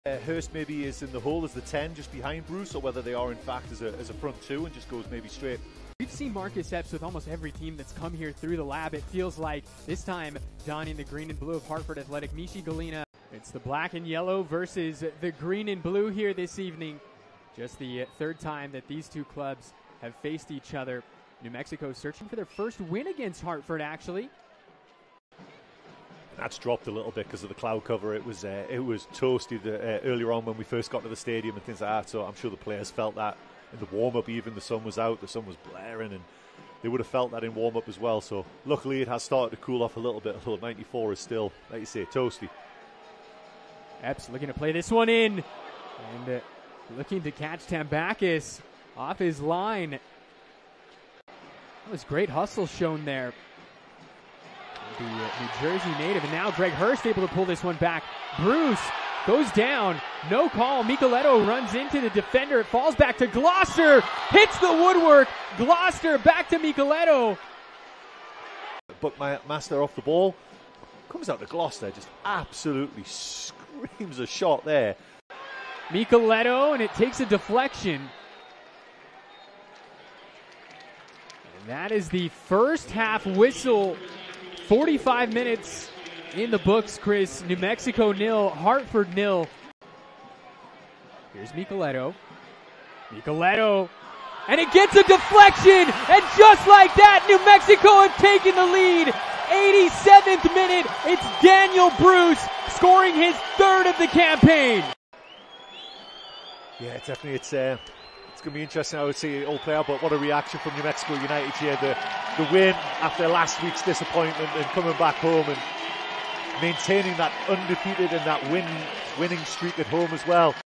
Hartford Athletic 0-1 New Mexico United | Highlights | USL Championship
Highlights of Hartford Athletic's 0-1 loss to New Mexico United